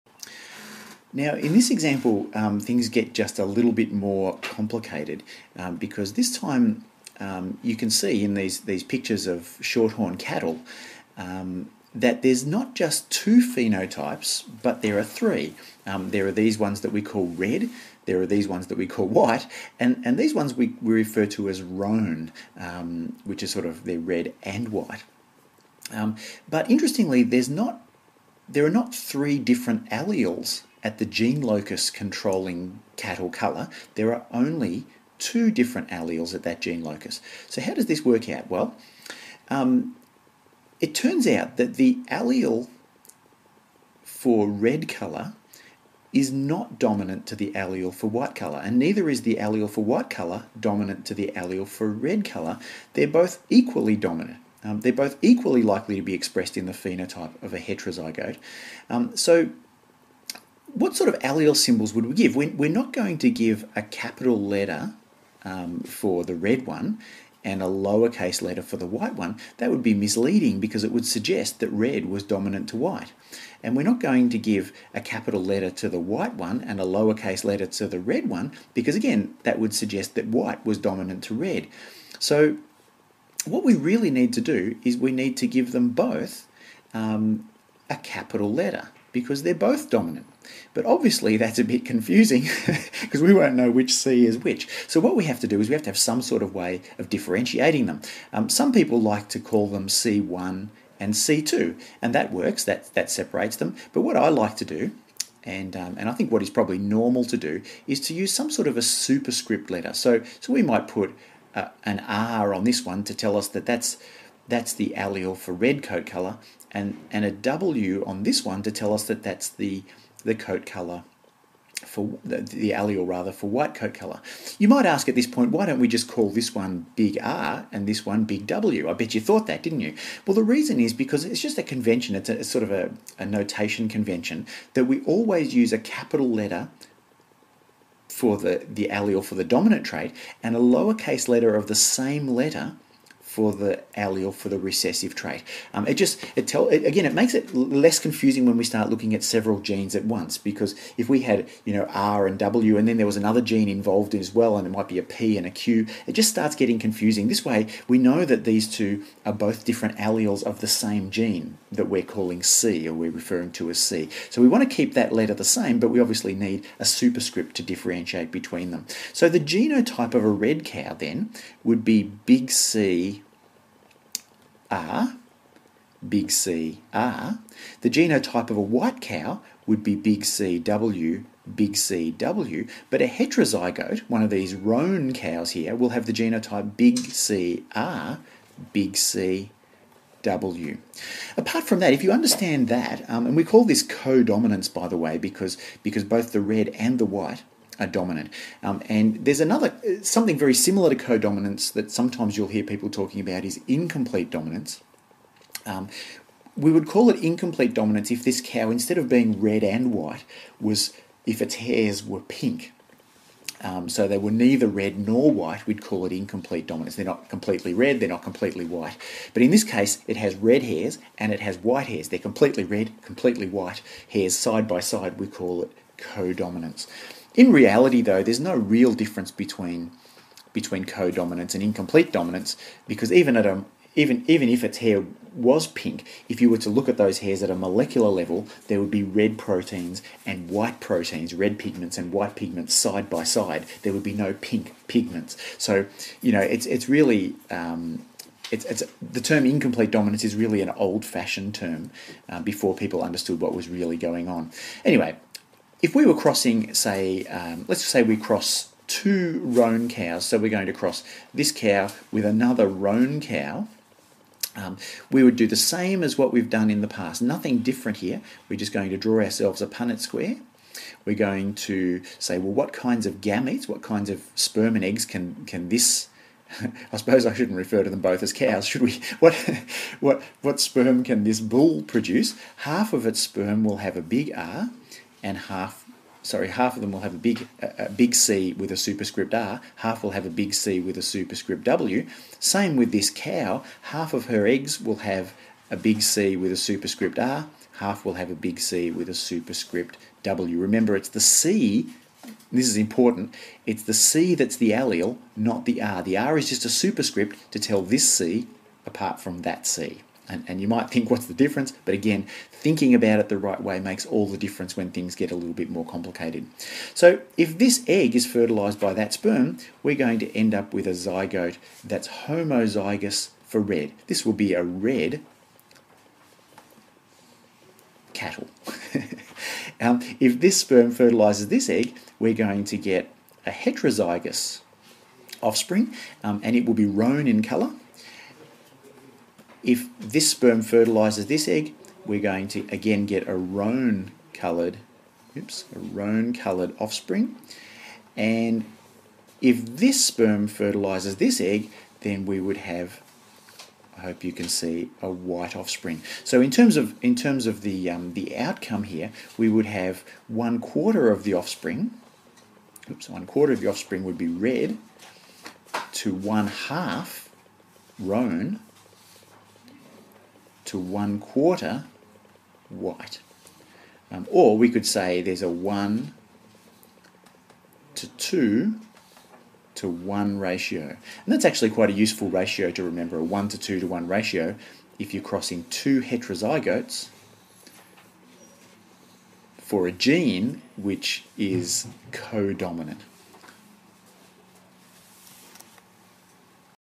Codominant Monohybrid Cross
This is about monohybrid crosses where the two alleles are codominant.